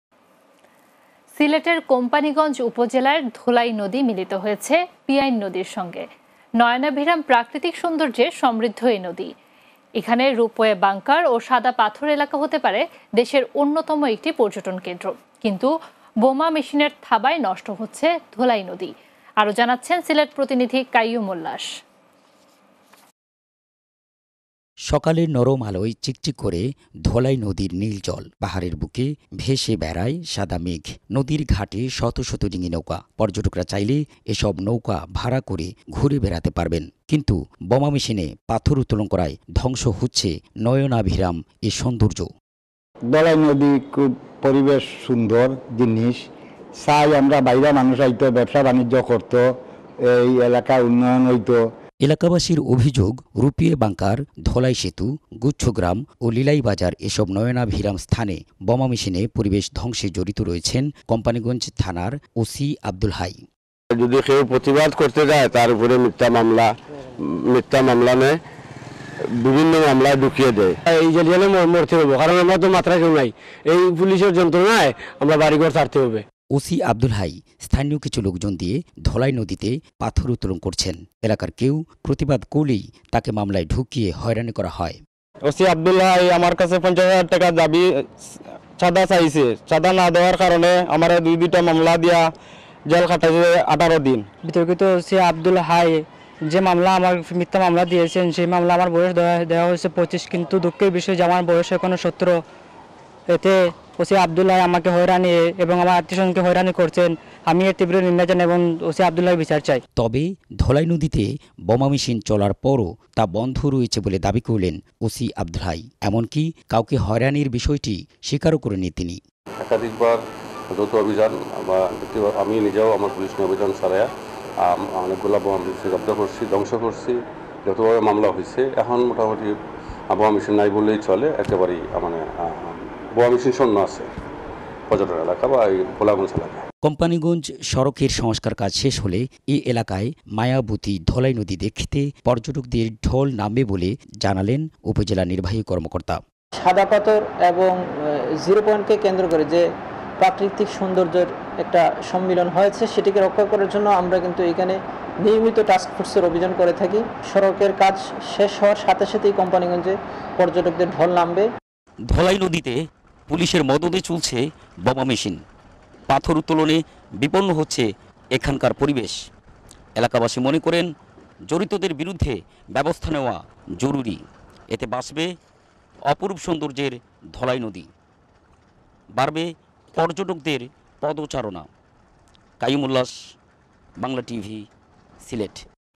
বোমা মেশিনের থাবায় ধ্বংস হচ্ছে ধলাই নদী
সিলেটের কোম্পানীগঞ্জ উপজেলায় ধলাই নদী মিলিত হয়েছে পিয়াইন নদীর সঙ্গে। নয়নাভিরাম প্রাকৃতিক সৌন্দর্যে সমৃদ্ধ এ নদী। এখানে রূপওয়ে বাংকার ও সাদাপাথর এলাকা হতে পারে দেশের অন্যতম একটি পর্যটনকেন্দ্র।

কিন্তু বোমা মেশিনের থাবায় ধ্বংস হচ্ছে ধলাই নদী। আরও জানাচ্ছেন সিলেট প্রতিনিধি কাইয়ুম উল্লাস।